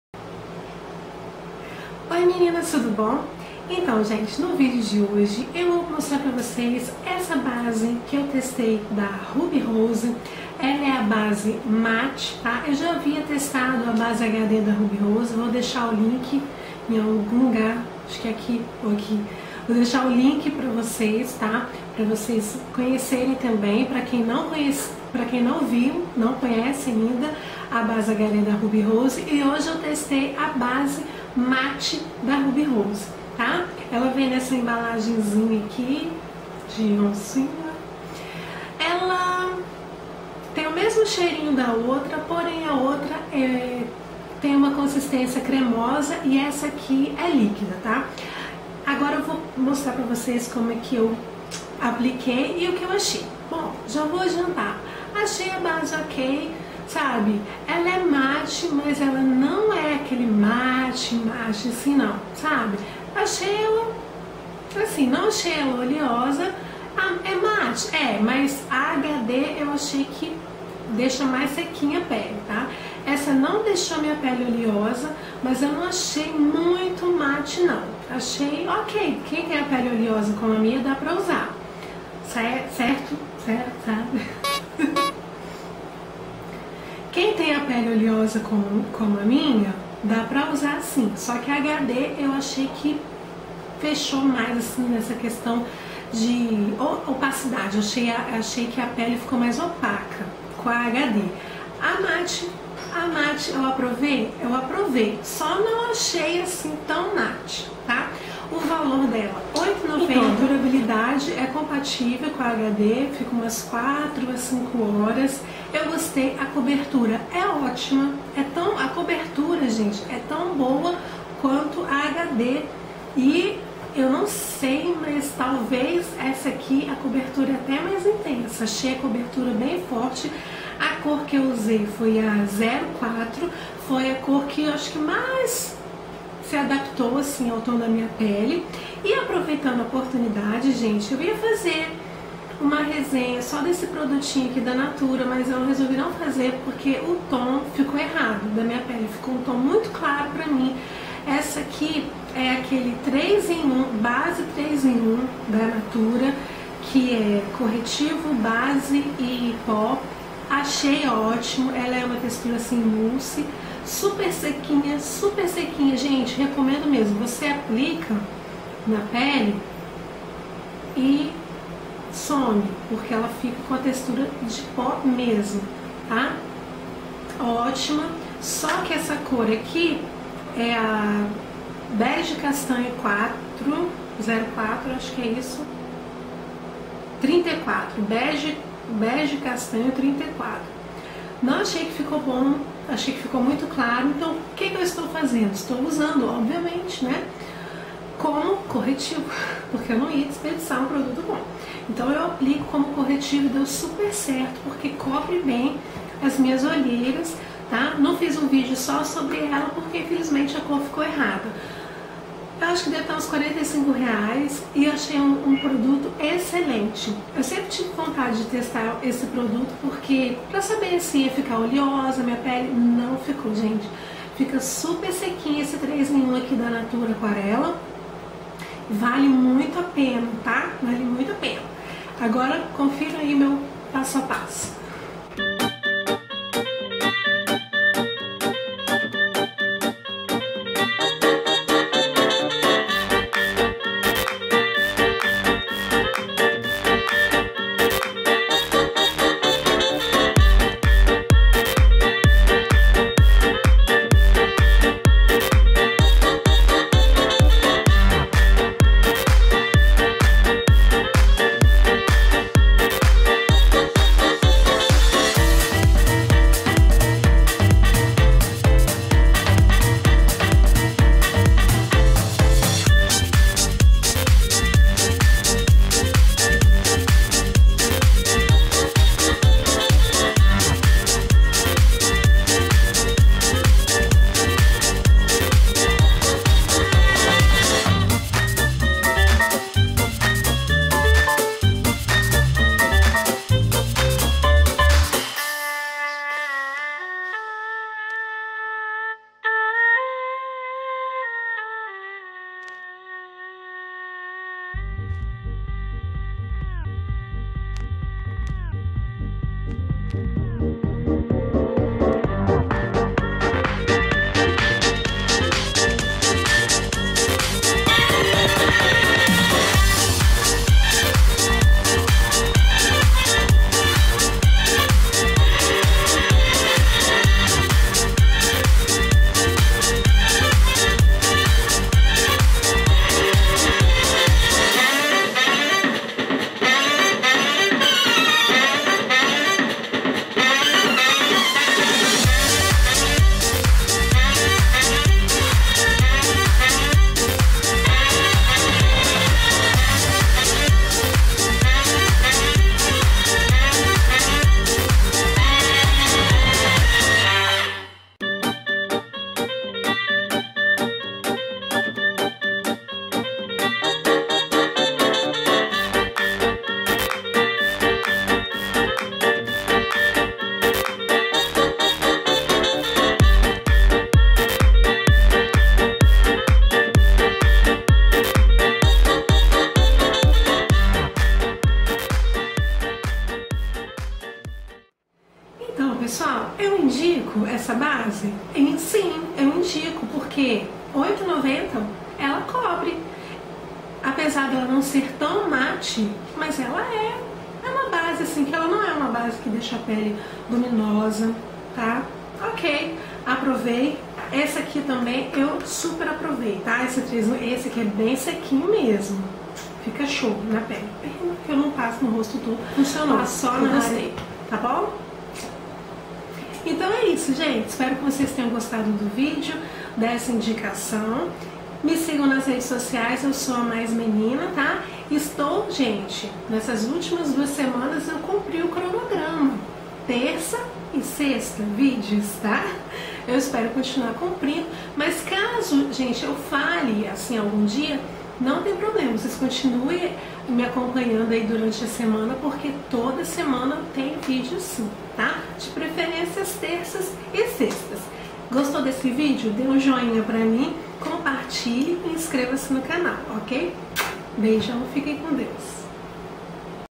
Resenha: Base Matte Ruby Rose e Base 3 em 1 Natura Aquarela
*ASSISTA EM QUALIDADE HD*
Testei a base Matte da Ruby Rose e a 3 em 1 da linha Natura Aquarela, dois produtos para cobertura de pele que são muito bons.